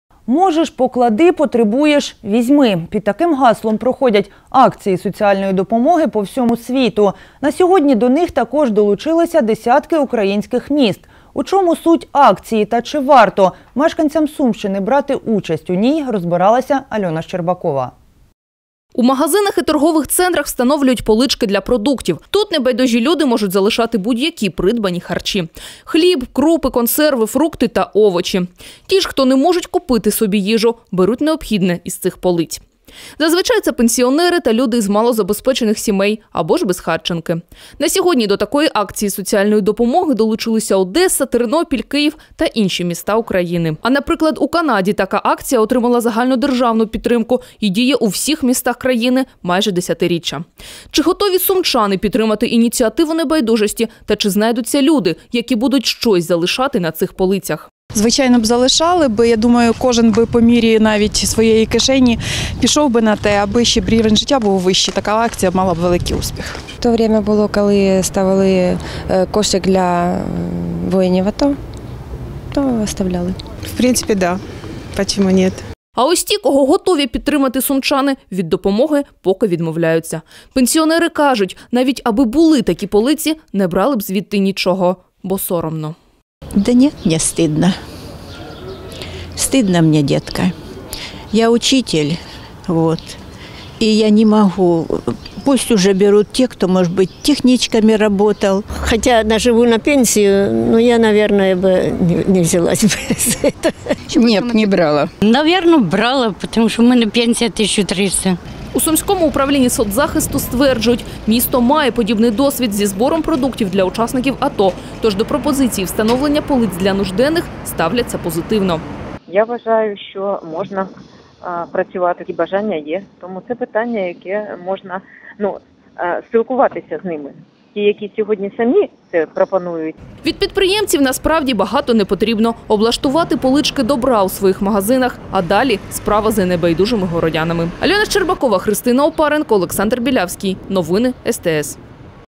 СТС шукає небайдужих на Сумщині: хто долучиться до соціального проекту?
Можеш – поклади, потребуєш – візьми. Під таким гаслом проходять акція соціальної допомоги по всьому світу. На сьогодні до неї також долучилися десятки українських міст. У чому суть акції та чи варто мешканцям Сумщини брати участь у ній – розбиралася Альона Щербакова.